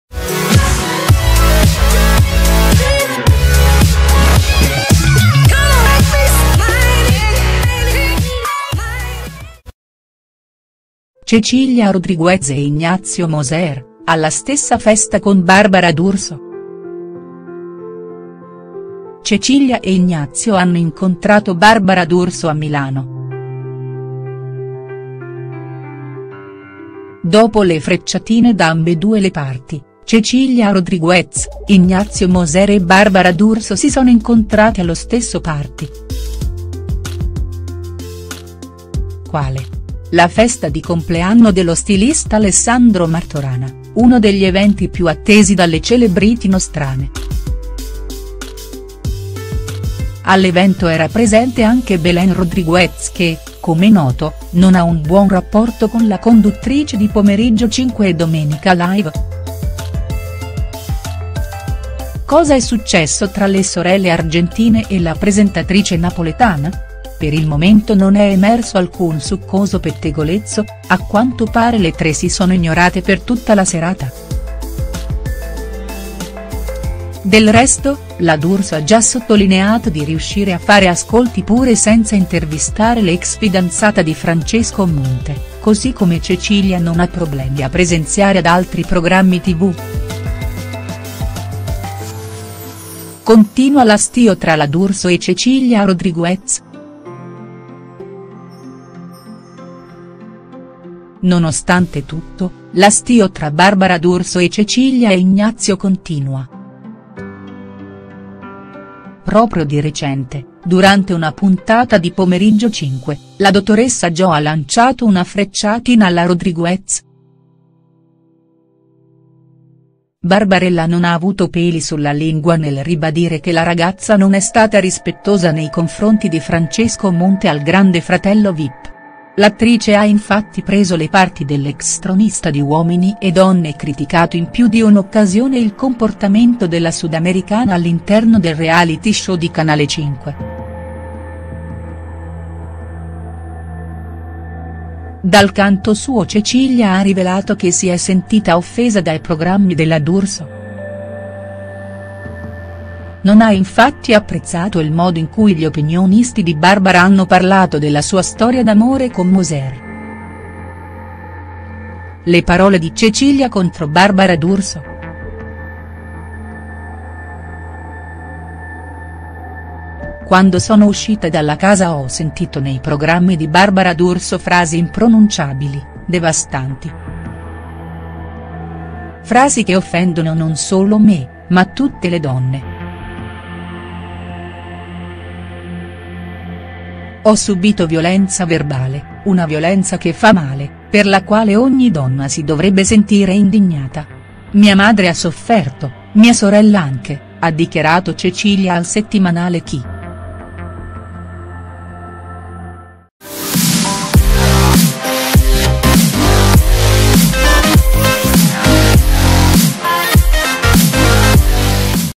Cecilia Rodriguez e Ignazio Moser: alla stessa festa con Barbara d’Urso| STARS NEWS
Cecilia e Ignazio dopo il Grande Fratello Vip hanno partecipato al party di Alessandro Martorana e lì hanno incontrato la loro rivale Barbara d'Urso.